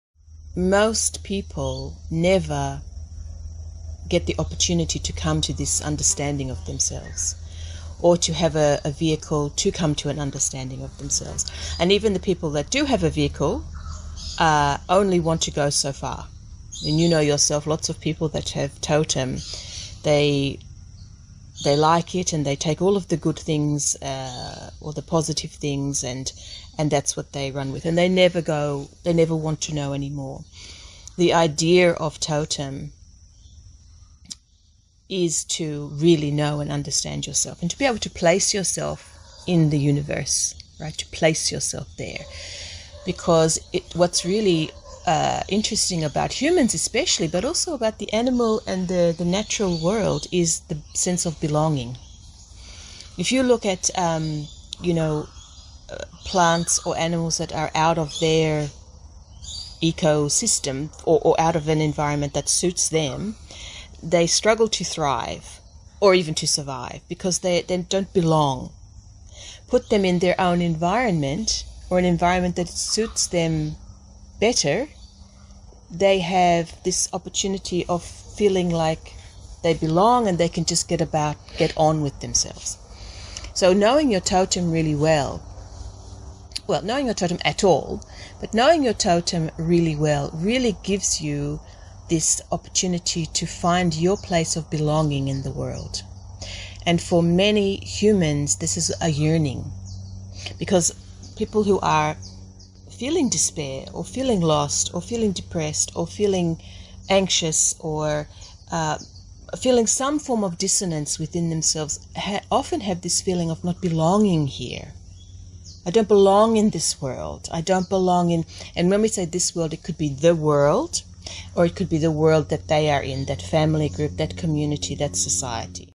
Totem - Finding your place in the world
Totem (Hethna) is who you are, the study of your nature and how that interacts with the universe. It explains the nature of your emotions, relationships, timing, needs, gifts and complications. It is you!
This is core work in our shamanic tradition.